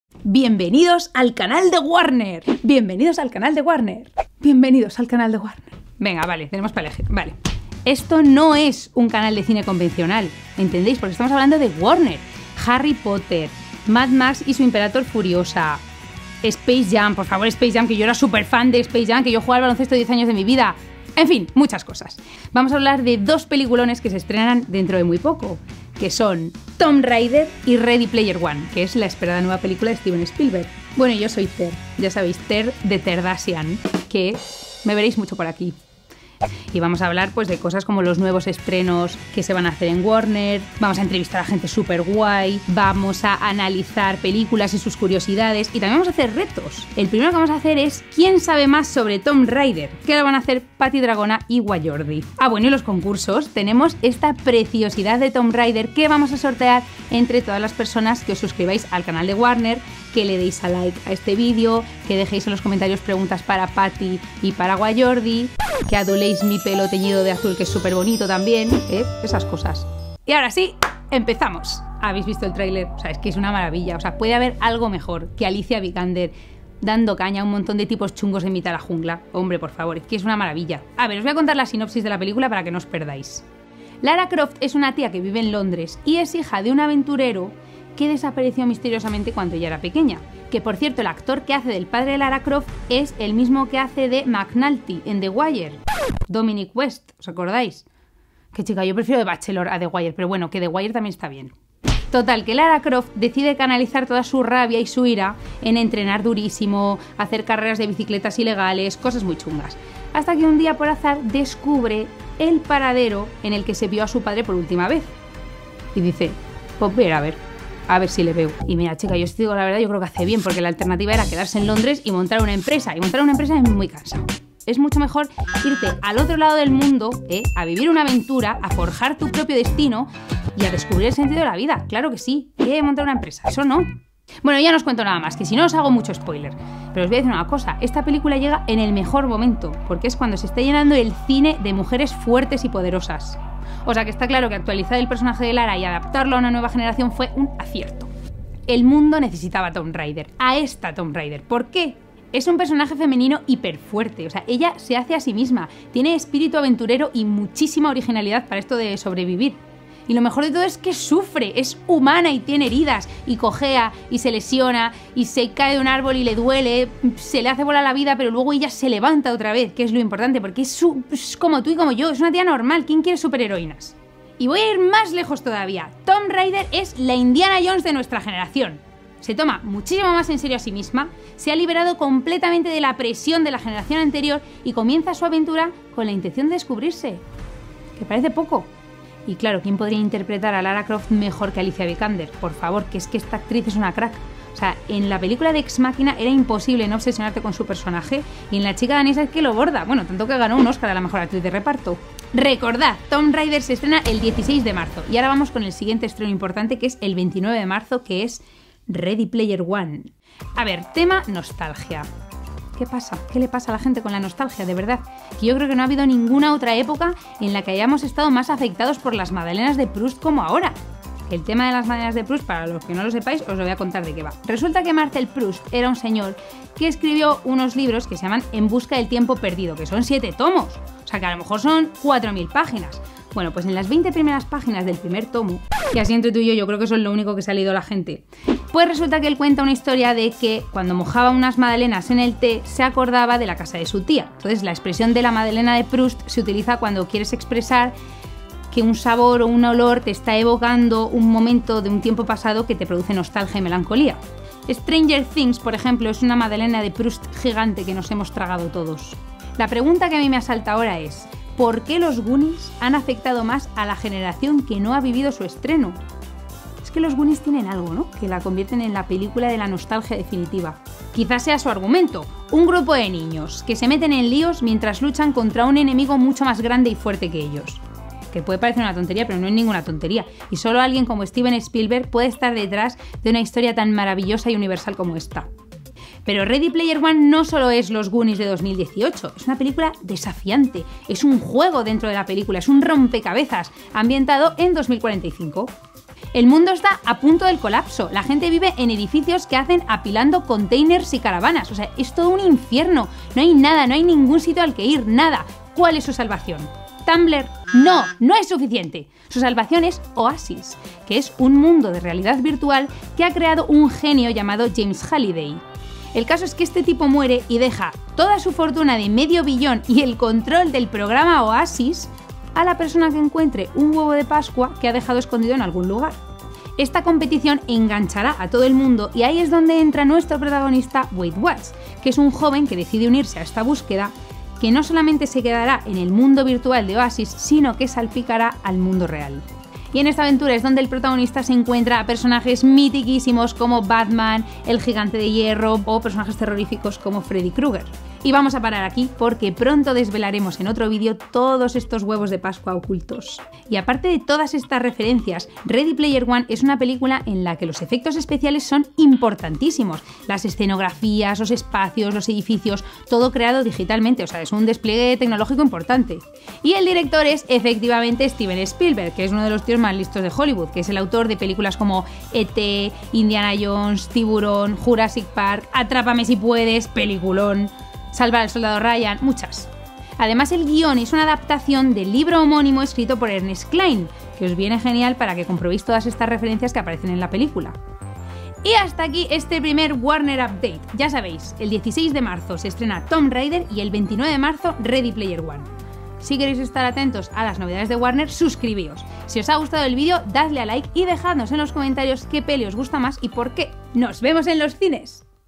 LARA CROFT es la heroína MILLENIAL - TER habla de TOMB RAIDER y READY PLAYER ONE
Hablamos de Lara Croft como la gran heroína de esta generación, de Alicia Vikander y de su gran trabajo en Tomb Raider. Te desvelamos las claves de Ready Player One y todo con la gracia e inteligencia de Ter.

Estreno de TOMB RAIDER: 16 de marzo de 2018

Estreno de READY PLAYER ONE: 29 de marzo de 2018